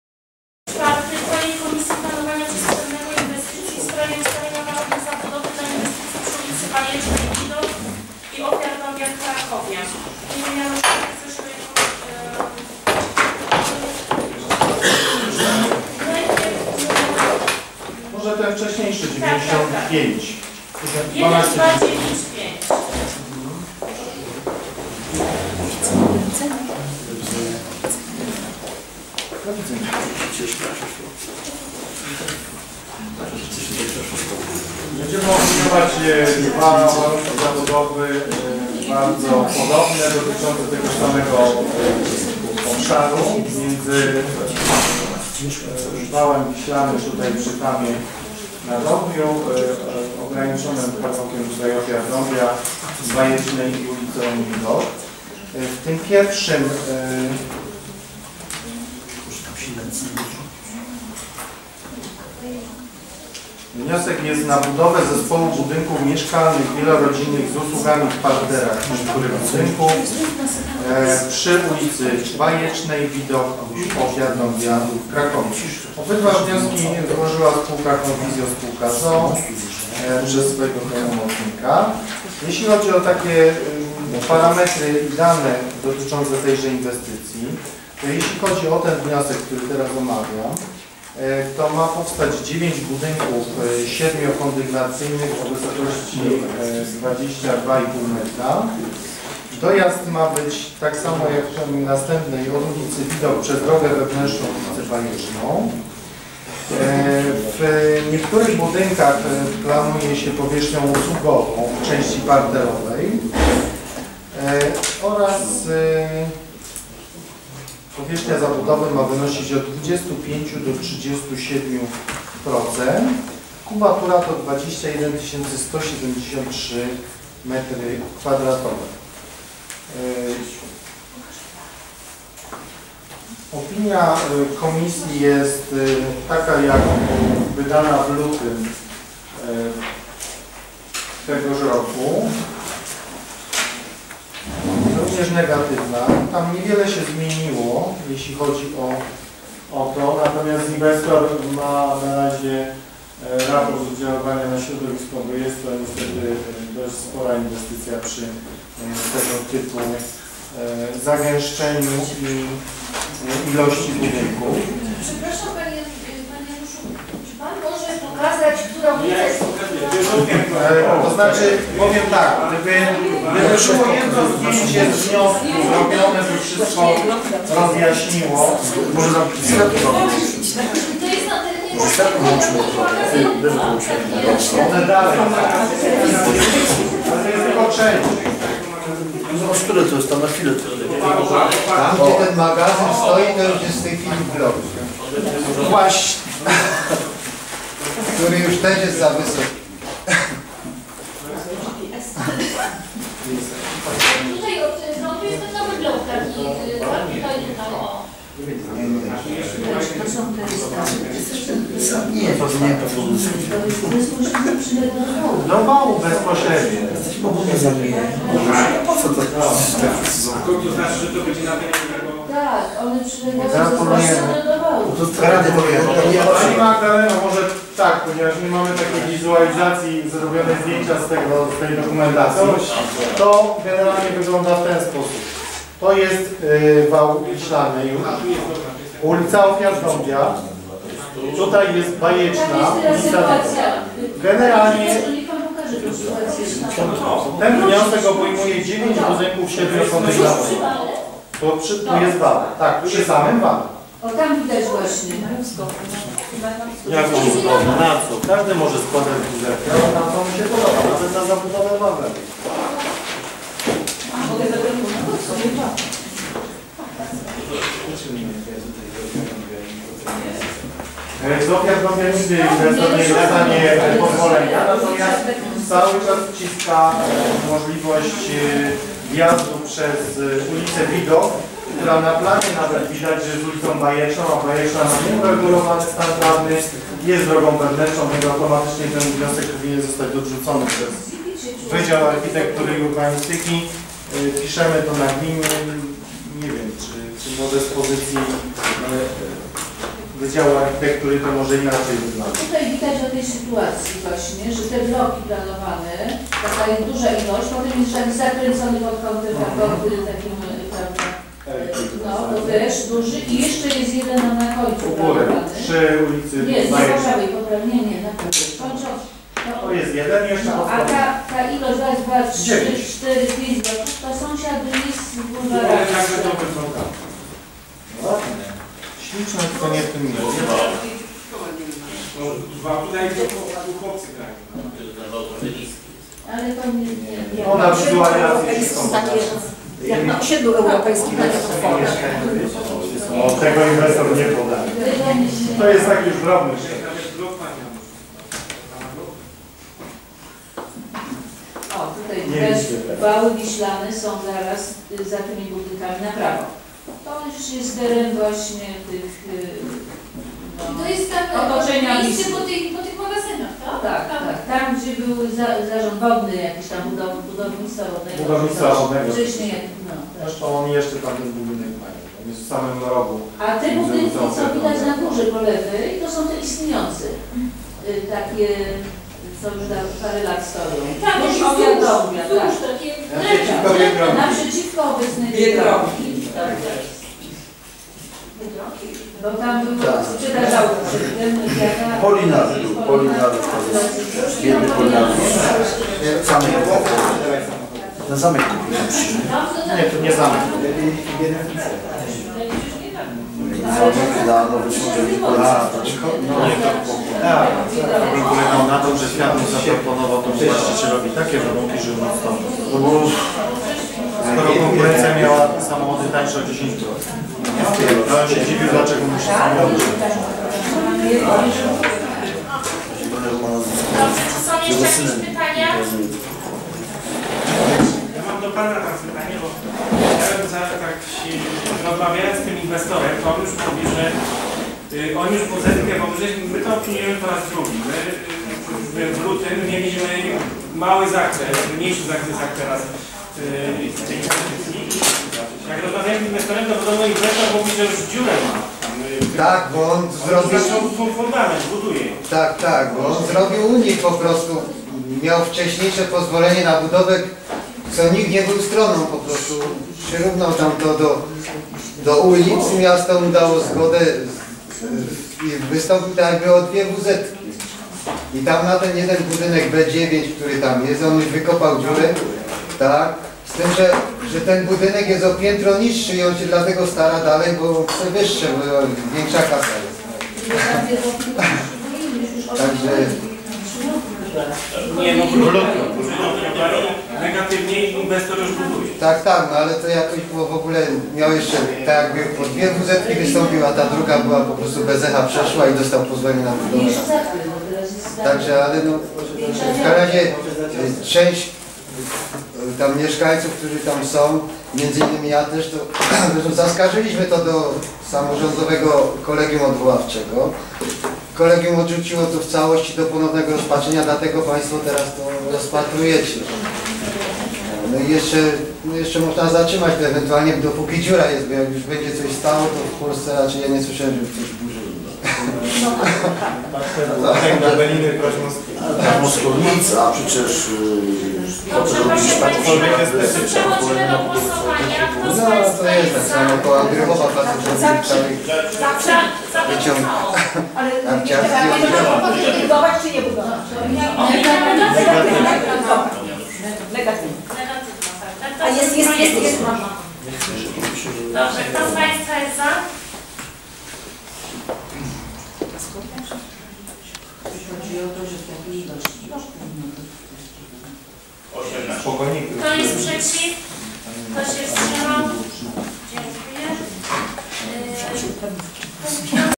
RD_II_18-10-2013_4 warunki zabudowy dla inwestycji przy ul.Bajecznej/Widok i Ofiar Dąbia
Obrady XXXIX sesji Rady Dzielnicy II Grzegórzki z dn. 18 października 2013 r. Uchwała w sprawie ustalenia warunków zabudowy dla inwestycji przy ul. Bajecznej/Widok i Ofiar Dąbia w Krakowie.